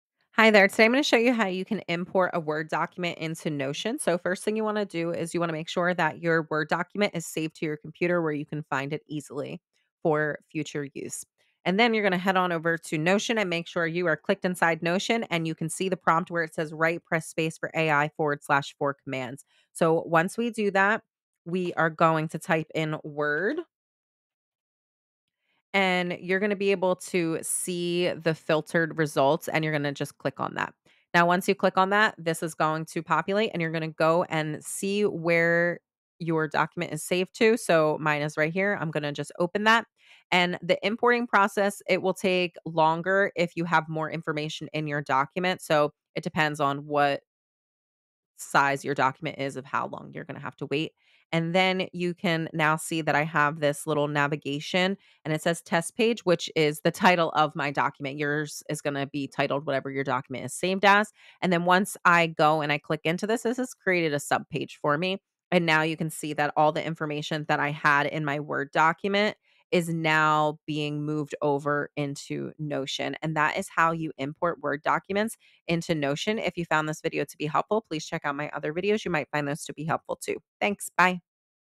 Notion Basics: Importing Word Documents
Welcome to my guide on importing Microsoft Word documents into Notion!
In this quick tutorial, I’ll walk you through how to bring your Word files into Notion so you can keep all your notes, documents, and projects in one place.

By the end of this video, you’ll know how to upload a Word document, view and edit its content directly in Notion, and organize it within your workspace.